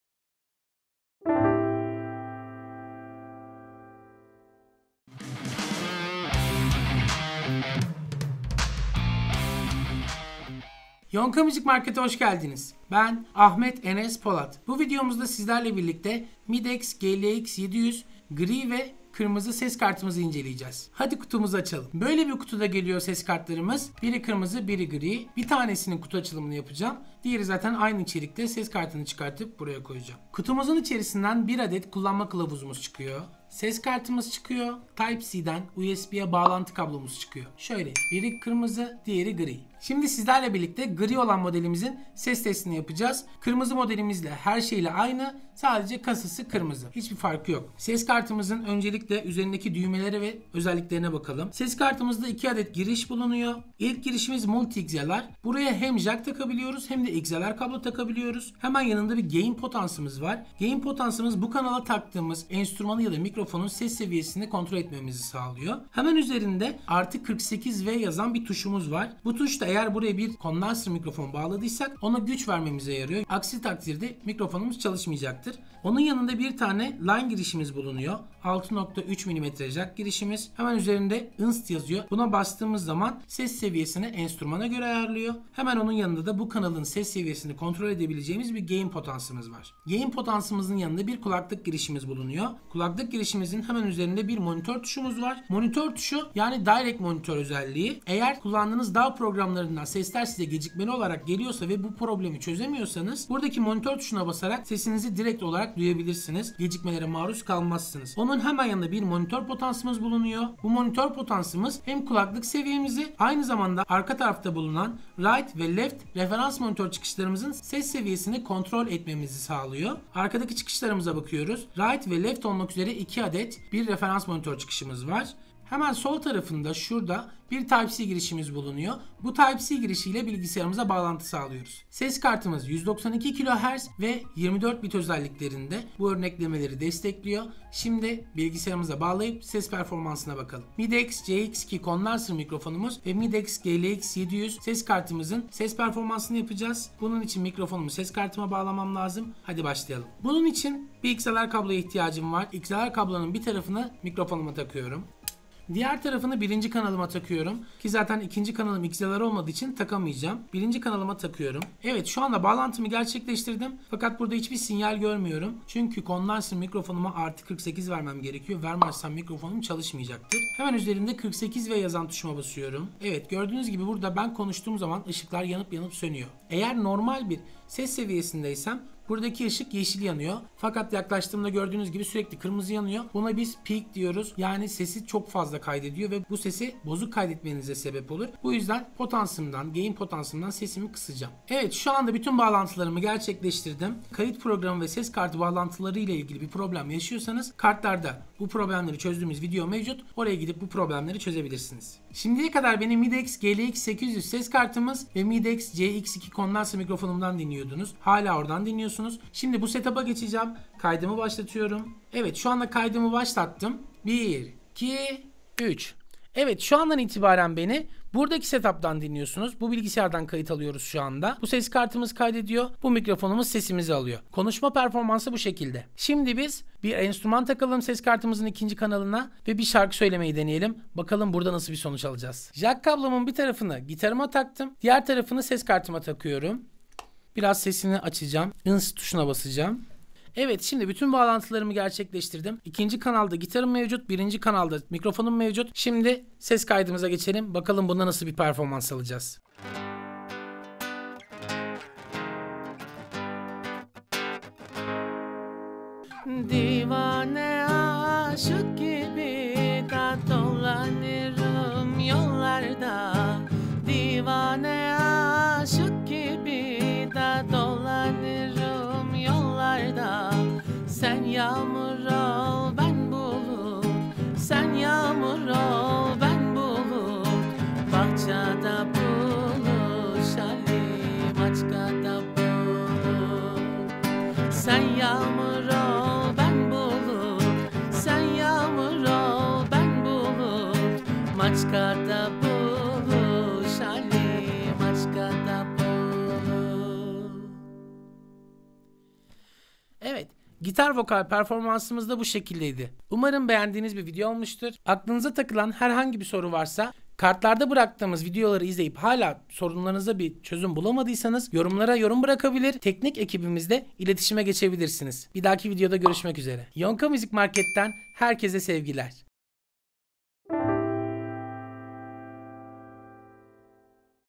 Midex GLX-700 Üst Segment Usb Stüdyo Ses Kartı 2 Giriş 2 Çıkış 24bit/192kHz
Midex ses kartı sayfamızda yer alan Midex GLX-700, 24bit/192kHz ve 2 giriş 2 çıkış profesyonel stüdyo ses kartı modelidir. Uygun ödeme seçenekleri ile Yonka'da

GLX-700 Özellikleri

24bit/192kHz
2 Giriş 2 Çıkış Stüdyo Ses Kartı
+48V Phantom Aç/Kapat
Dinamik Aralık: 95dB
Maksimum Giriş Seviyesi: +8 dBu
Frekans Tepkisi: 20 Hz - 20 kHz ±1 dB
Gürültü Engelleme: -110 dBu
Gain Tepkisi: 50 dB
Mikrofon Giriş Empedans: 3 kohms
Enstrüman Giriş Empedans: 1 Mohms
Line Girişi: 20 kohms
Kulaklık Çıkış Empedansı: 30~300ohm
Monitör Çıkış Empedansı: 221 ohms

Giriş ve Çıkışlar:
1 AD. Mikrofon Girişi (6,3 mm Jak ve 3 Pin XLR)
1 AD. Enstrüman Girişi (6,3 mm Mono JAK)
1 AD. Kulaklık Girişi (6,3 mm Stereo JAK)
1 AD. Referans Monitörü Çıkışı (2x6,3 mm Mono JAK)
1 AD. TYPE C Bilgisayar Aktarım Çıkışı (Bilgisyar Girişi USB)

Ebatları ve Ağırlık:
Genişlik: 140 mm
Uzunluk: 109 mm
Yükseklik: 38 mm
Ağırlık: 337 gram

Garanti ve Teslimat Süresi

Garanti Süresi: 2 YIL
Kargo ücreti: Ücretsiz Kargo

Whatsapp Satış & Destek : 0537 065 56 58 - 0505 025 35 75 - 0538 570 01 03
Çağrı Merkezi : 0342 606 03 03